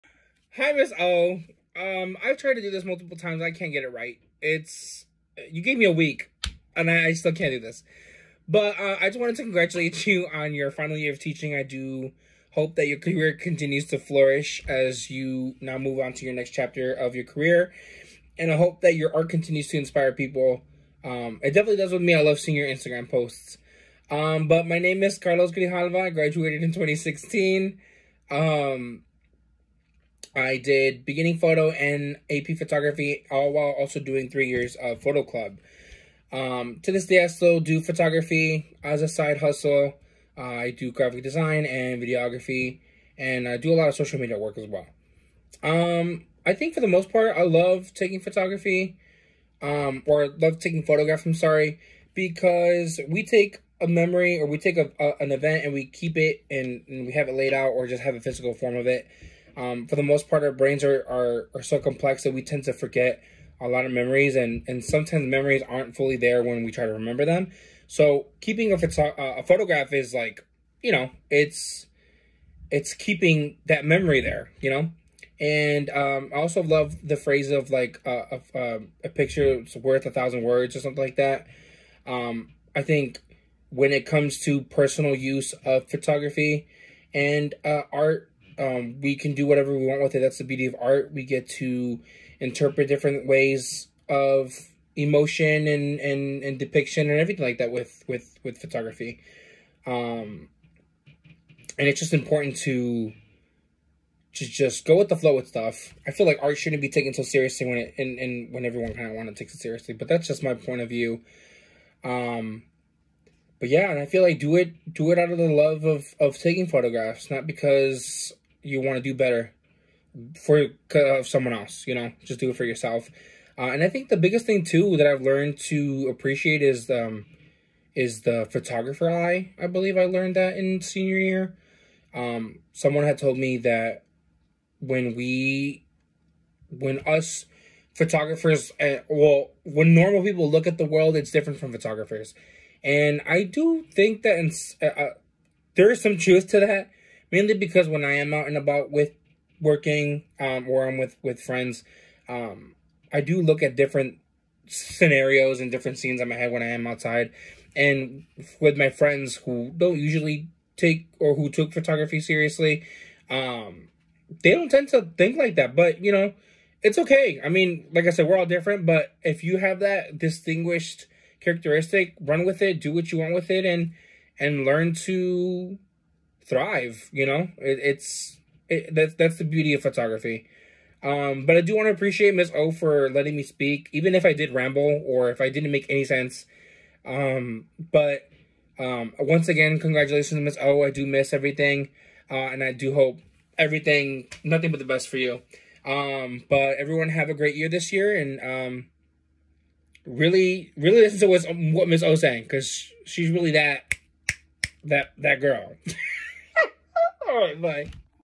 Carlos Grijalva on why he takes photos and what photography means to him.
I asked some former students to help inspire the 23-24 Morton West Photography students. Each person was asked to make a video about why they take photographs and what photography means to them.